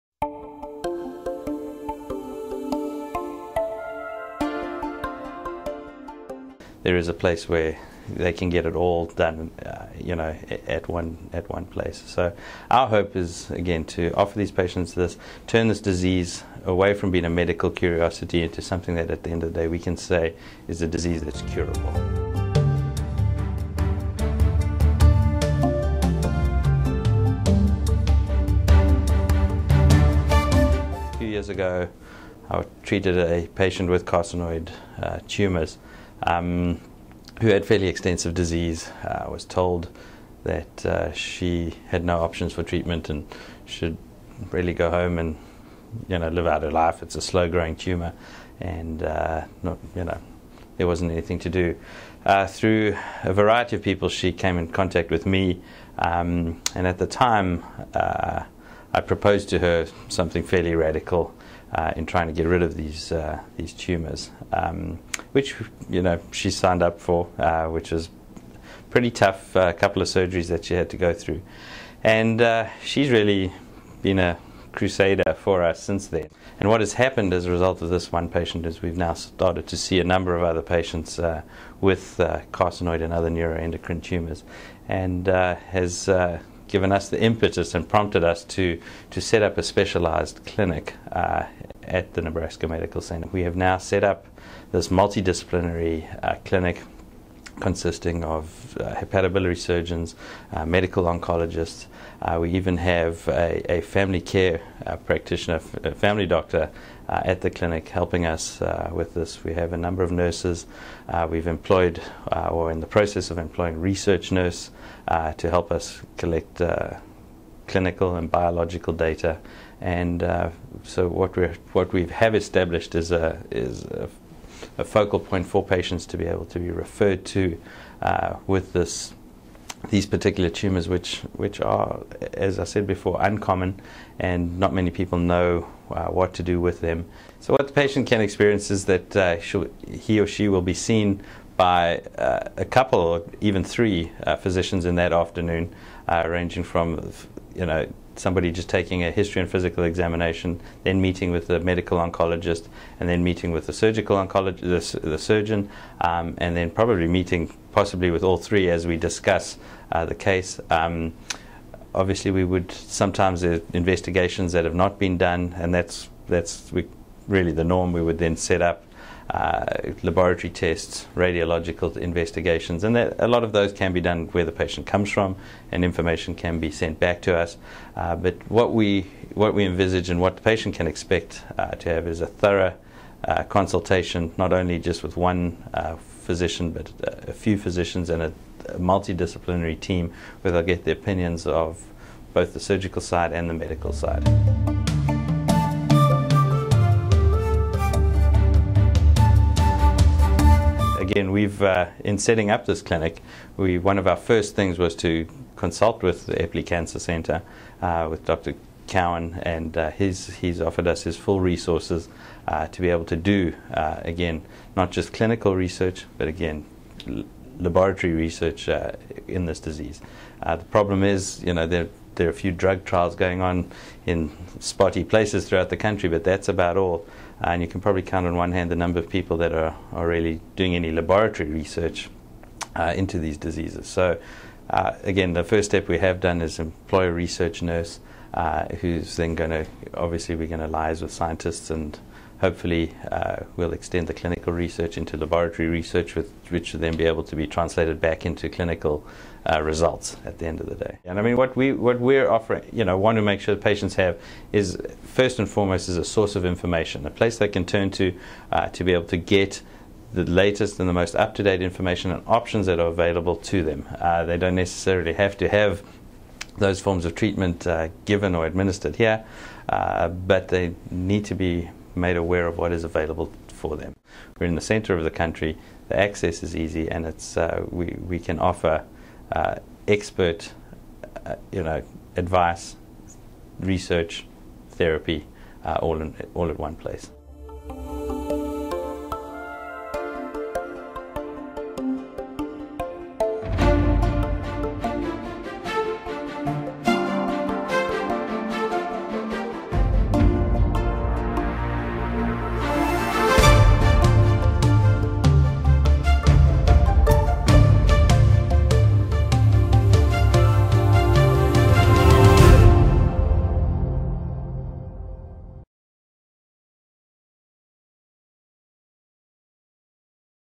Carcinoid Clinic - The Nebraska Medical Center
A multi-disciplinary team of physicians at The Nebraska Medical Center started the Carcinoid Clinic as a way to take the disease from medical curiosity to curable condition.

The Carcinoid Clinic at The Nebraska Medical Center offers patients quality and comprehensive care.  Patients will meet doctors with a variety of specialties during their clinic visit.  Patients at the clinic can expect to consult with a medical oncologist, surgeon and even a family practice physician.  In addition to providing treatment to patients, the carcinoid clinic will also have a significant role in research.  A grant from the NU Foundation provided money for the clinic to hire a research nurse who will work closely with the multidisciplinary physicians.  The physicians involved with the clinic believe The Nebraska Medical Center and UNMC is the ideal place for such a clinic.  The medical center is home to an internationally known liver and intestinal transplant program and the UNMC Eppley Cancer Center.  The Eppley Cancer Center is recognized by the National Cancer Institute (NCI) and is a founding member institution of the National Comprehensive Cancer Network (NCCN).  Doctors hope the medical center's reputation for patient care and research, along with a convenient geographic location in the center of the U.S. will make visiting the clinic a realistic option for patients from all over the country.  or by calling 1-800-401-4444.